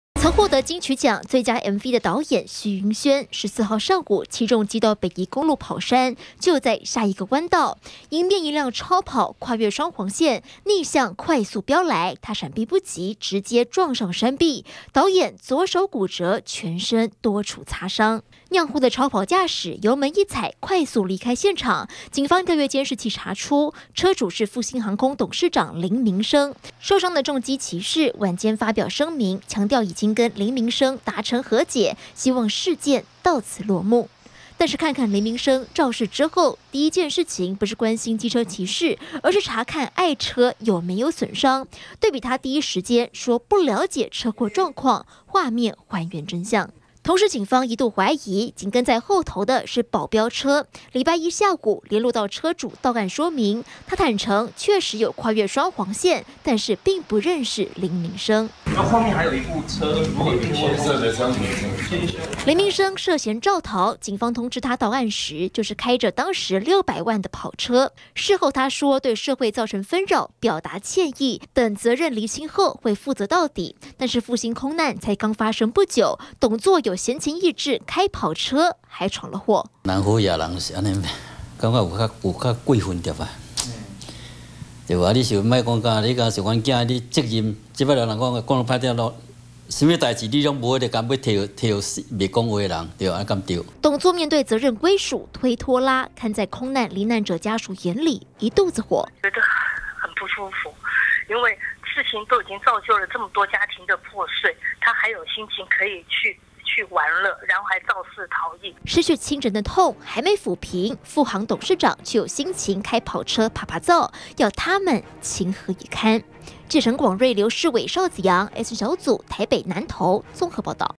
20150317中天新聞 不知發生車禍？ 林明昇曾下車查看
持續追蹤復興航空董事長林明昇涉肇逃事件，今天的最新進度是警方約談了當時一起違規逆向的廂型車車主，前來說明，而這名駕駛供稱，因為看到前方保時捷逆向超車，才跟著違規並非與林明昇認識，而摔車的導演，徐筠軒，也在晚間發表聲明，表示整起事件是誤會，雙方並未發生擦撞，並已和解，期盼事件能夠平安落幕。